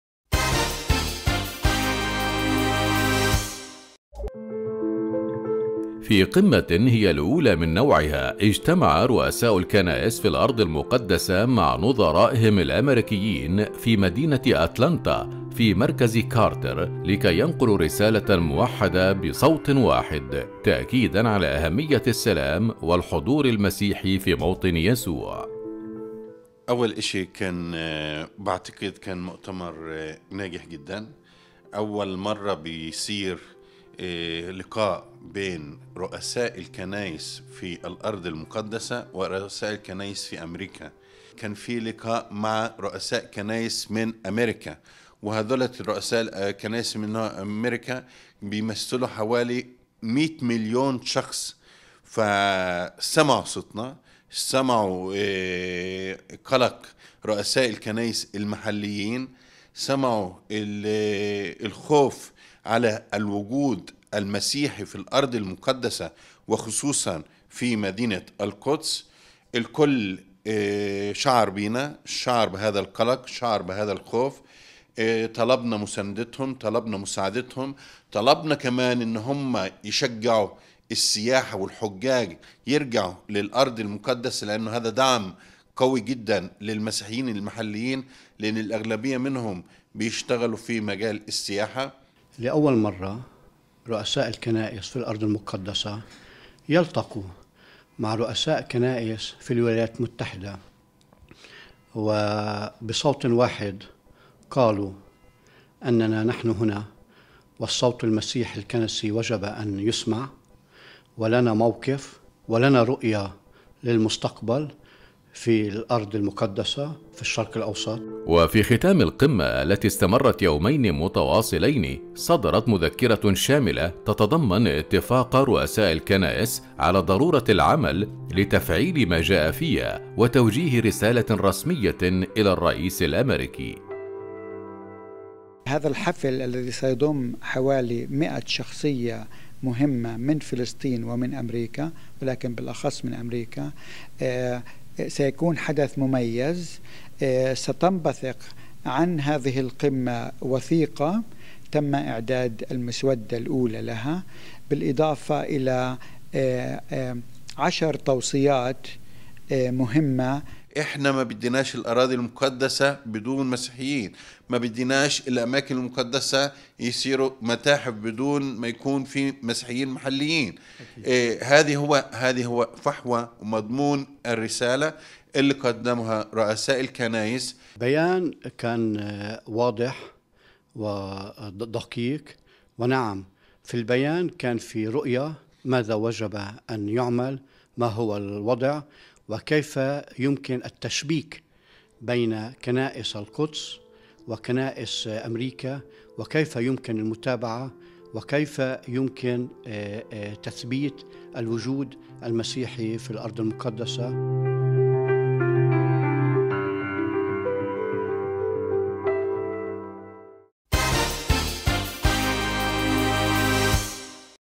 مؤتمر قمة يضم رؤساء الكنائس في الأرض المقدسة والولايات المتحدة
تحت شعار "تحقيق السلام وتعزيز الحضور"، انعقد يوم التاسع عشر من شهر نيسان الماضي في مدينة أتلانتا الأميركية في مركز كارتر أول مؤتمر قمة لرؤساء الكنائس في الولايات المتحدة والأراضي المقدسة بحضور الرئيس الأميركي الأسبق جيمي كارتر.